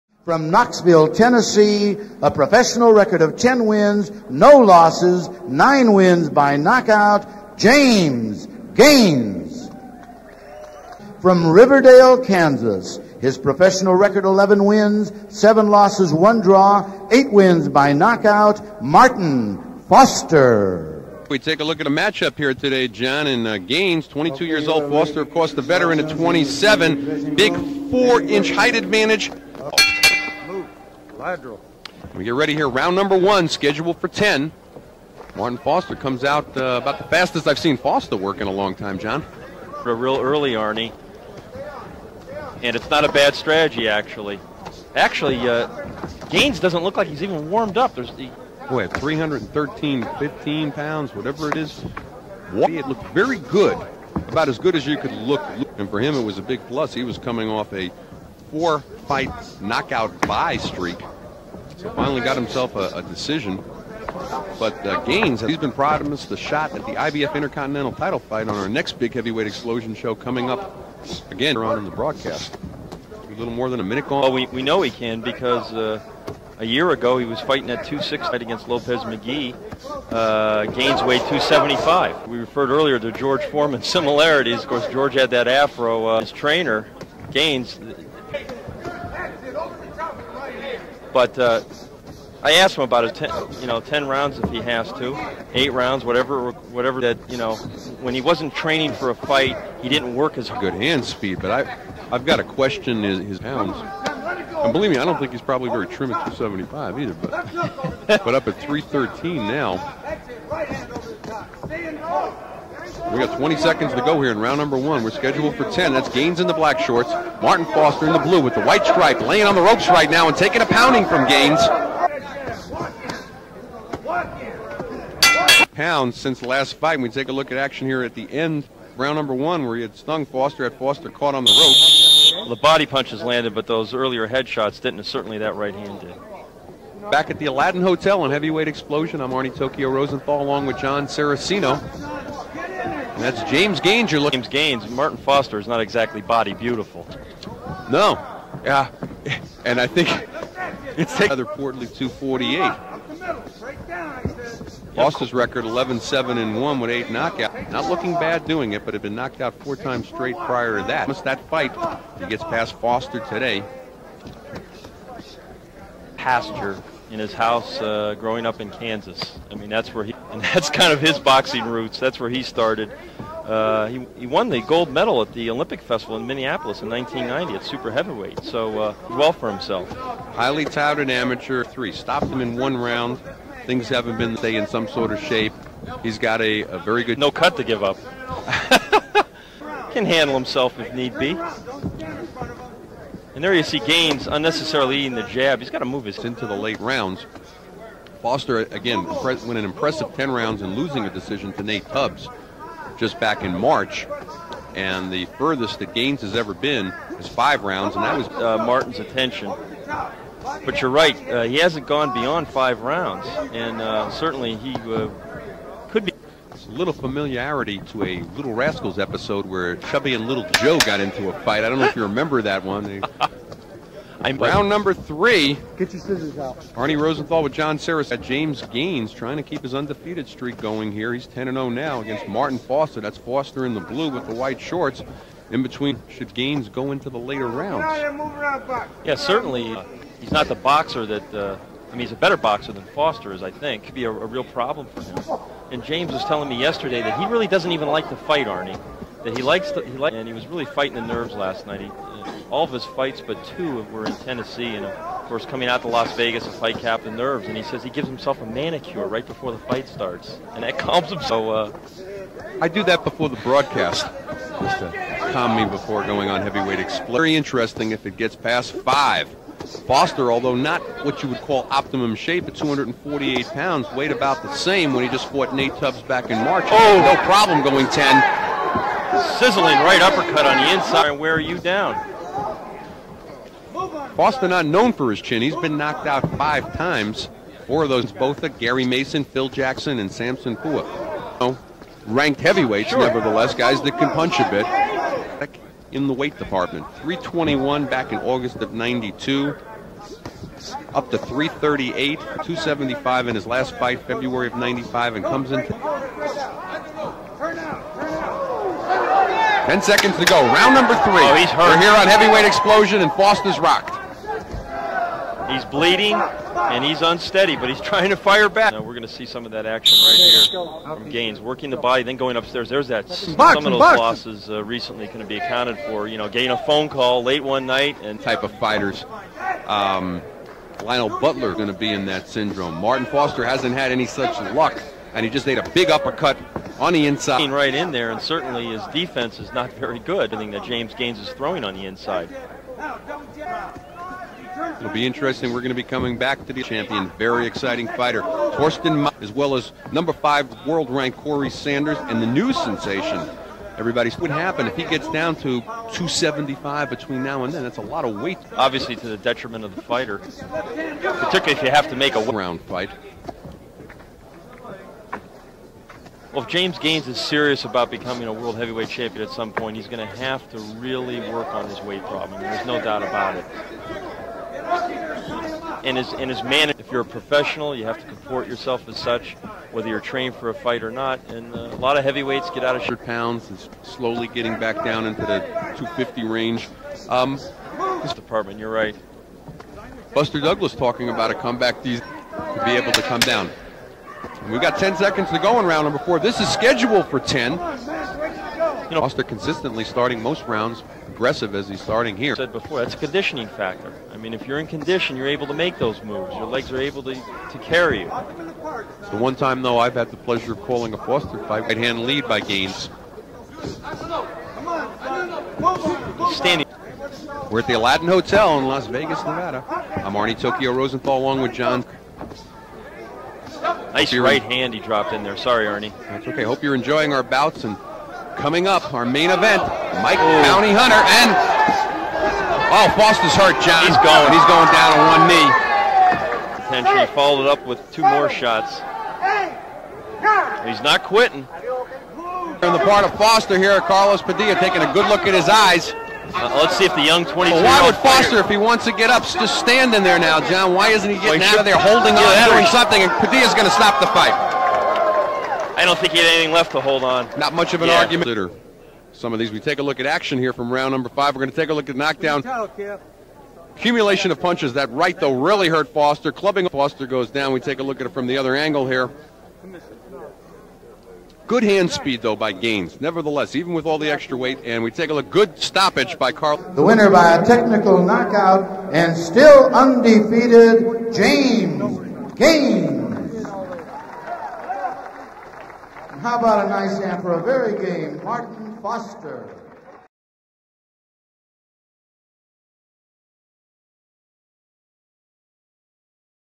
James Gaines vs Martin Foster // Highlights
In a heavyweight clash, James Gaines achieved a technical knockout against Martin Foster in the fifth round of their bout on May 6, 1995. The event took place at The Aladdin in Las Vegas, pitting two American boxers in a contest scheduled for 10 rounds. Gaines, showcasing his orthodox stance and reach, delivered a decisive victory in this professional match refereed by Carlos Padilla.